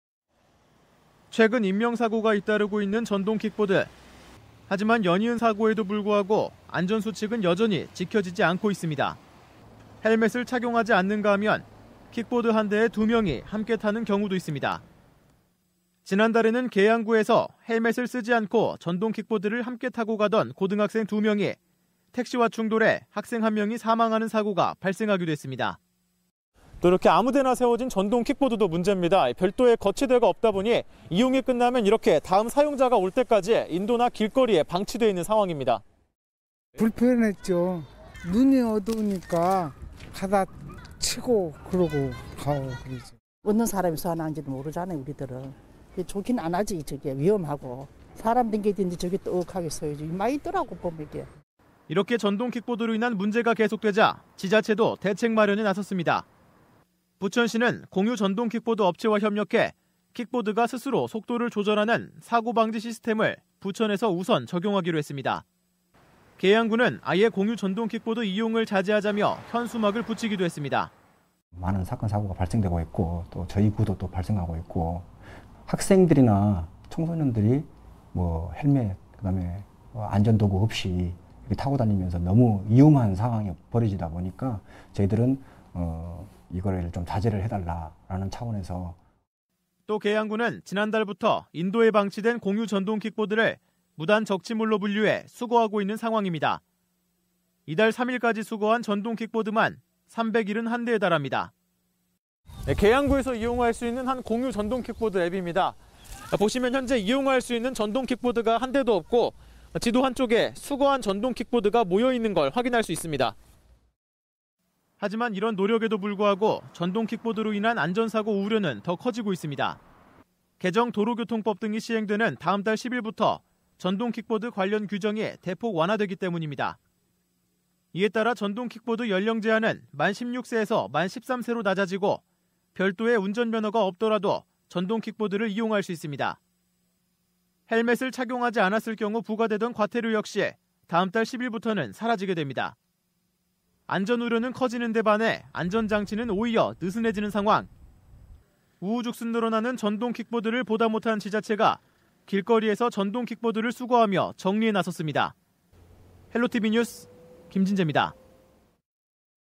전동킥보드 사고 잇따라... 지자체 수거 나서
얼마 전 계양구에서 전동킥보드를 타던
고등학생이 택시와 충돌해 사망하는 등
전동킥보드로 인한 사고가 계속해서 발생하고 있습니다.
하지만 여전히 길거리에서는
안전 수칙이 제대로 지켜지지 않고 있는 상황인데요.
보다 못한 지자체가
급기야 인도에 무단으로 세워진 전동킥보드를 수거하고 나섰습니다.
김진재 기자입니다.